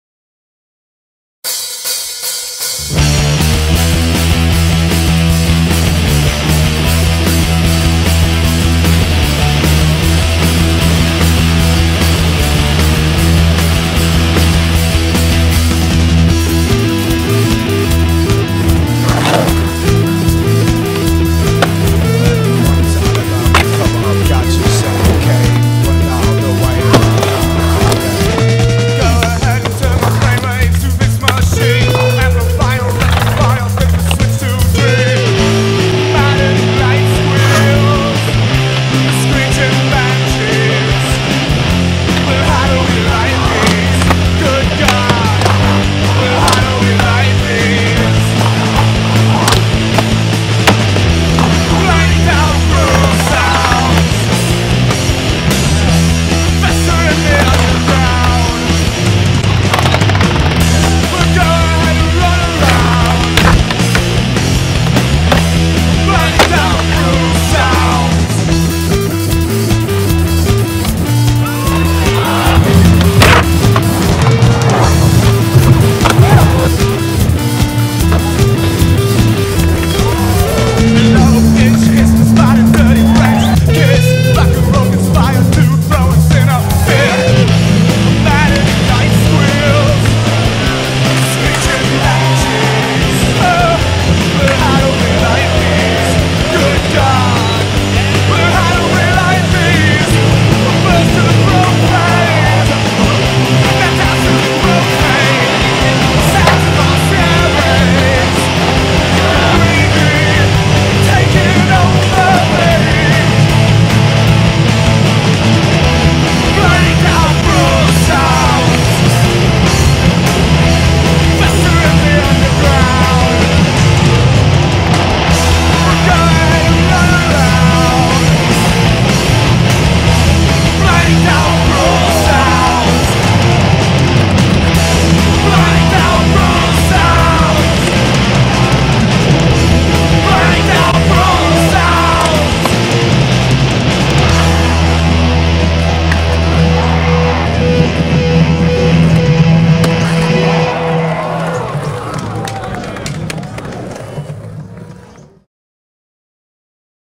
Skatepark Of Tampa Concrete Courtyard- Tampa AM 2014
Filmed by- Frank Branca & Kevin Perez
Edited by-Kevin Perez

Here's some of the amazing skating that went down on the new concrete courtyard we built over at the Skatepark Of Tampa during the 21st Annual Tampa Am 2014.